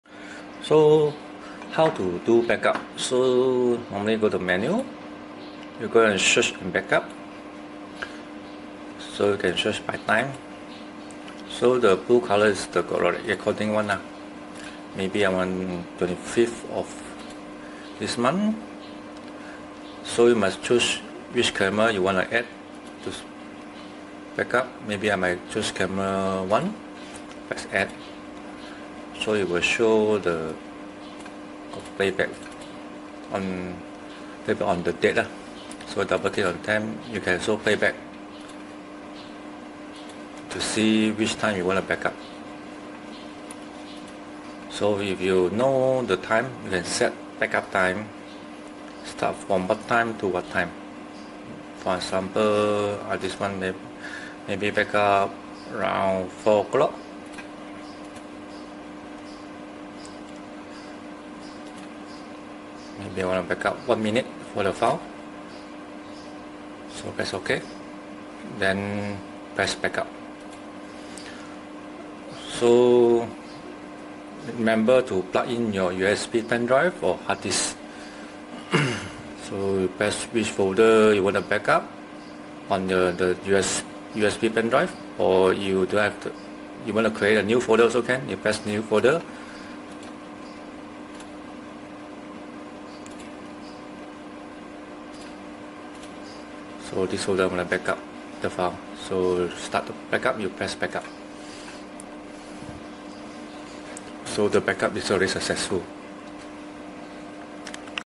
SouthTech TVT DVR/NVR (Apps: SuperLivePlus) How to do back up file and copy to thumb drive
You can copy out important part video to thumb drive and playback in other device example proof of staff sleep in store room :(

Menu - Search and Backup - Press by Time - Bottom + Add Which Camera You Want to Backup
- Choose Which Date - Set Backup Time - Start Time and End Time - It Will Show How Long Is the Backup Process and Use How Many Space - Press Backup (Remember to Plug in Your Pen Drive) - Save It on Your Pen Drive
(Choose Avi Format) Then Press Backup